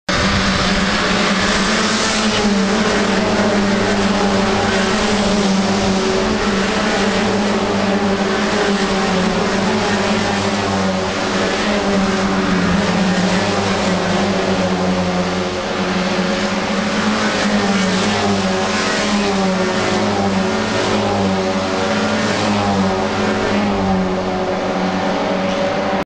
Spec Miata NASA Race at Mid Ohio 2012
Video of one of the re-starts from the Spec Miata Class race at Mid Ohio, 09/08/12.